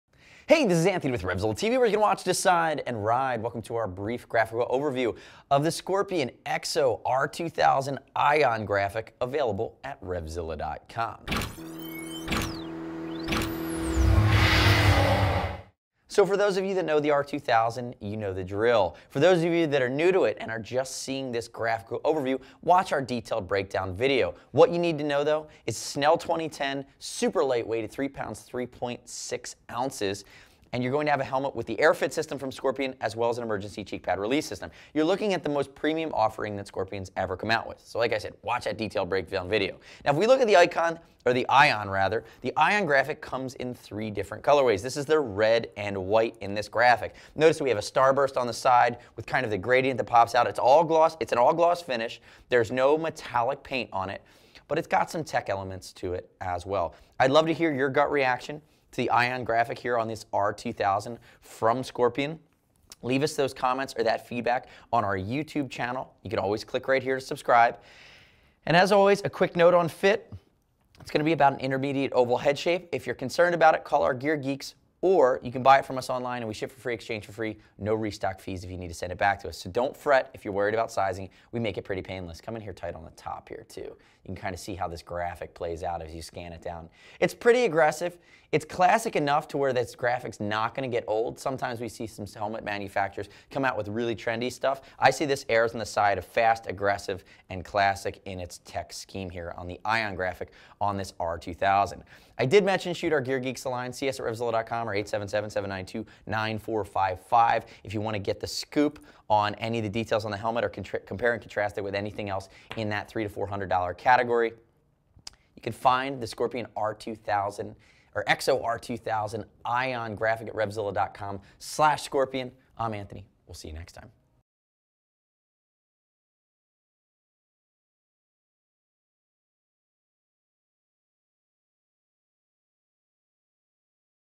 Scorpion EXO-R2000 Ion Helmet Review at RevZilla.com
Scorpion EXO-R2000 Ion Helmet Review
The Scorpion EXO-R2000 Ion helmet is a graphic on the new top-end offering from Scorpion. The Ion graphic is a futuristic design that uses bold contrasting graphics to create layers of dimensions on the helmet. Offered in Red, as shown in this video, as well as Blue and Matte Silver options. For a full detailed breakdown on the Scorpion EXO-R2000, check out this video